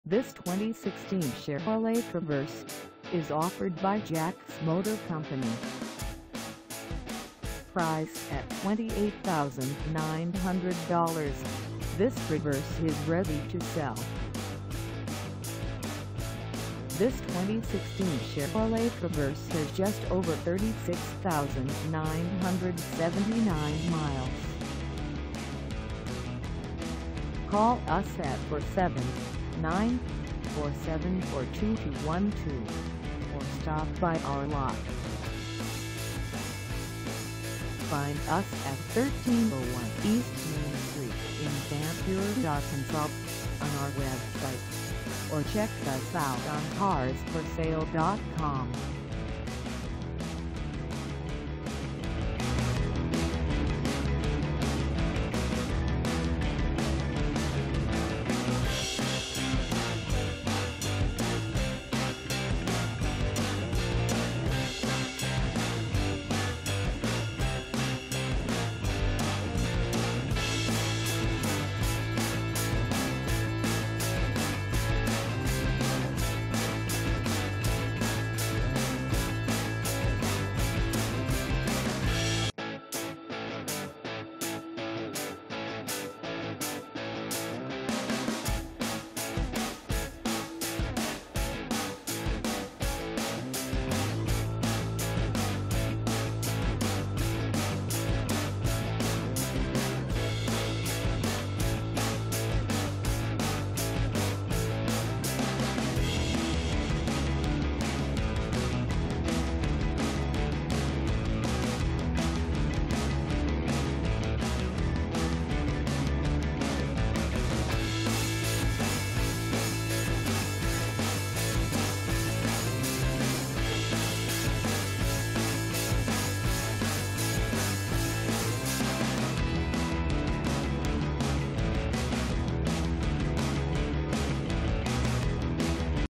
2016 Chevrolet Traverse LT AWD 4dr SUV w/2LT for sale in Van
This 2016 Chevrolet Traverse LT AWD 4dr SUV w/2LT is for sale in Van Buren, AR 72956 at Jacks Motor Co.

Engine: 3.6L V6
Transmission: Automatic 6-Speed
Color: Black
Mileage: 36979
Contact Jacks Motor Co 1301  East Main St Van Buren, AR 72956 to test drive this 2016 Chevrolet Traverse LT AWD 4dr SUV w/2LT today.